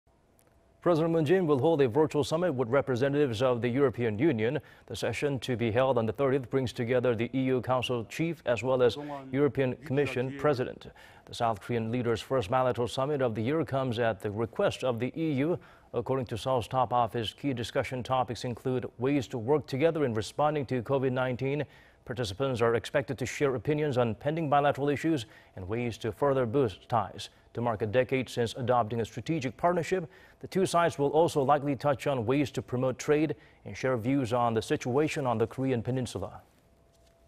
South Korea, EU to hold virtual summit on June 30
문대통령, 30일 한-EU 화상 정상회담

President Moon Jae-in will hold a virtual summit with representatives of the European Union.
The session to be held on the 30th,  brings together the EU Council chief as well as and European Commission President.
The South Korean leader's first bilateral summit of the year comes at the request of the EU.
According to Seoul's top office, key discussion topics include ways to work together in responding to COVID-19.
Participants are expected to share opinions on pending bilateral issues and ways to further boost ties.
To a decade since adopting a strategic partnership, the two sides will also likely touch on ways to promote trade, and share views on the situation on the Korea Peninsula.